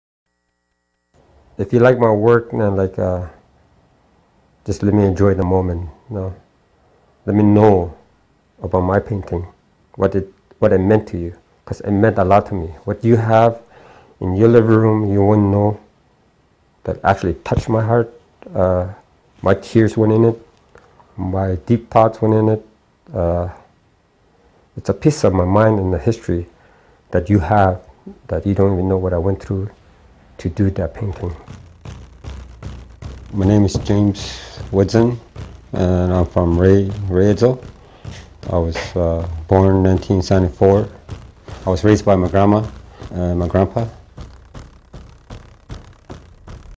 From the Spirit: James Wedzin
James Wedzin from Behchoko (Fort Rae), Northwest Territories, praises his grandmother for inspiring and encouraging his early interest in traditional art. He watched her create intricate bead work endlessly and developed his own sense of art style. James is known for his northern landscapes, animal imagery and northern lights.